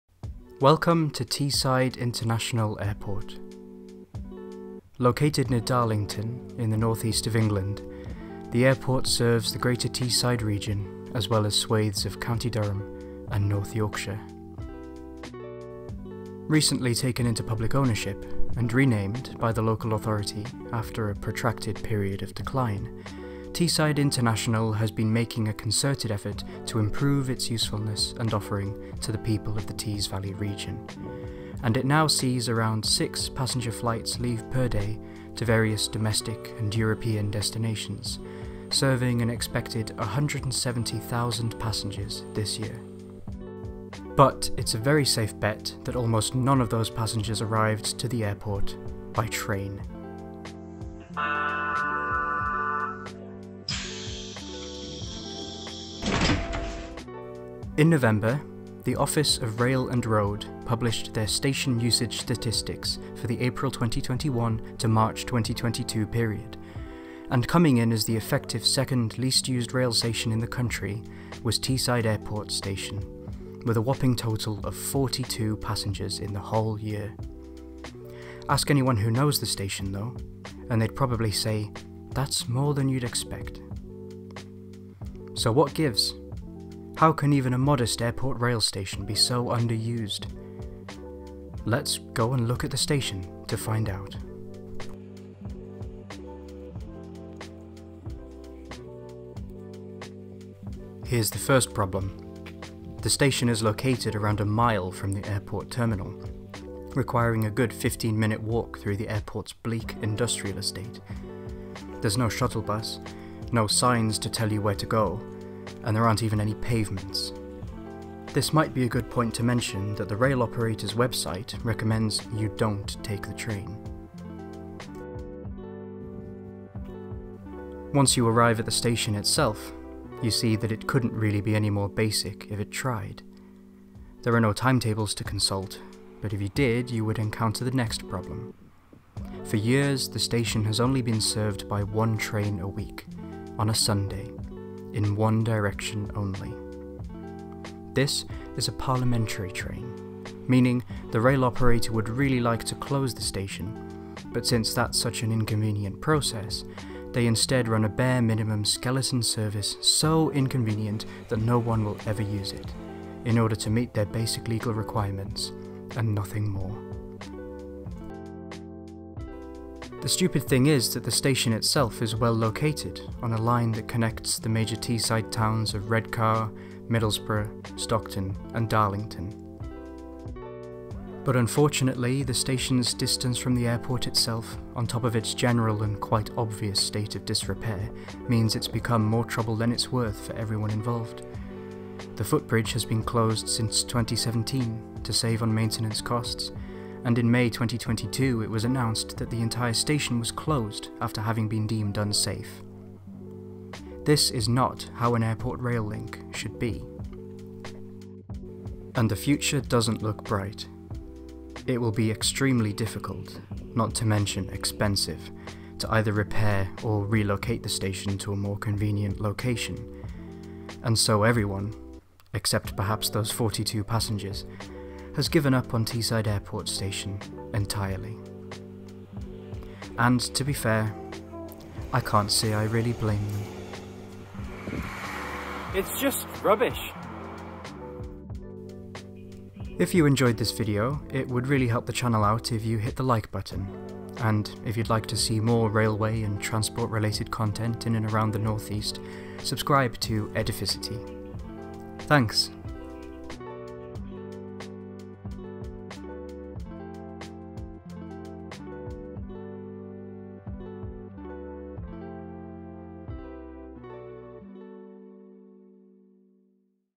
The Most USELESS Airport Station in Britain?
If an airport is close to a train station, you would be forgiven for thinking you could actually use it when you're taking a plane. But not here... welcome to Teesside Airport station, officially one of the least used train stations in Britain.

Let's find out why it's just so useless.